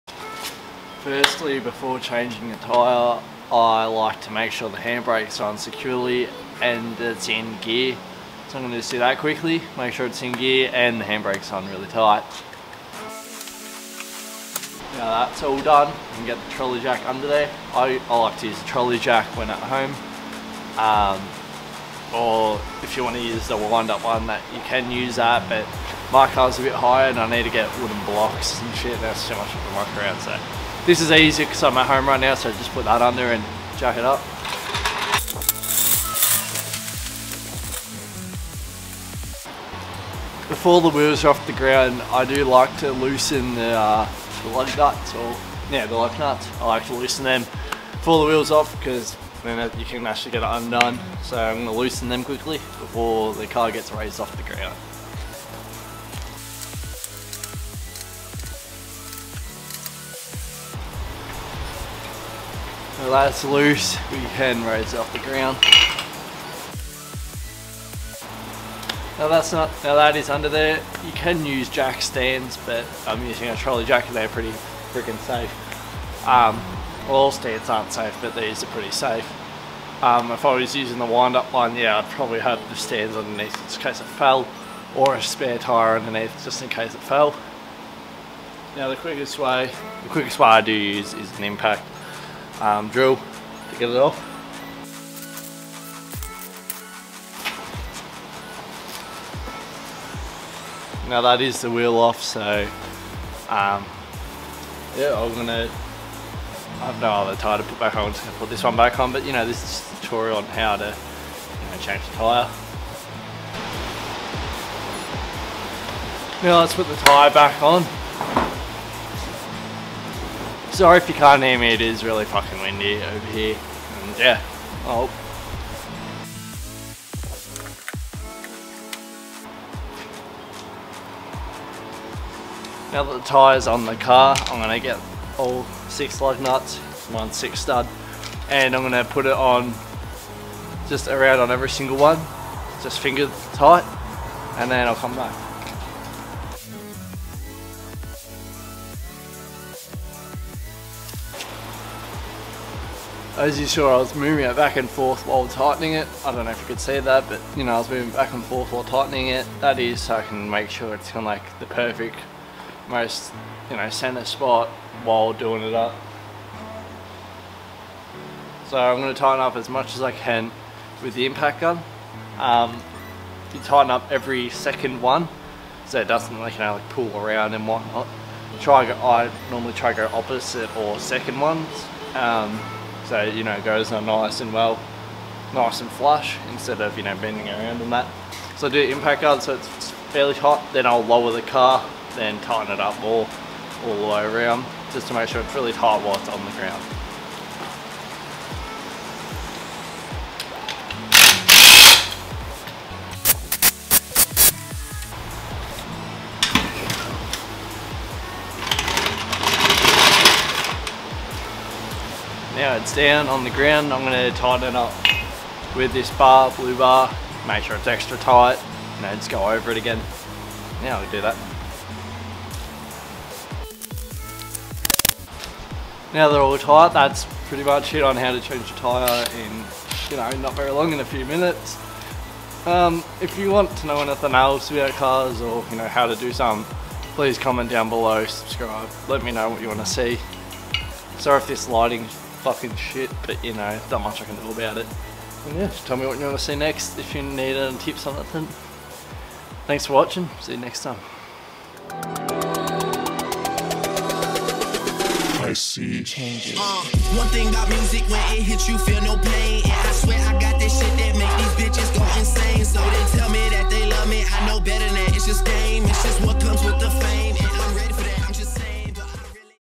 How to Change a Tire (less than 5 minutes)
This is how to change a tire, the right way, and the quick way under 5 minutes!! EASY tutorial.
JarrodPaynee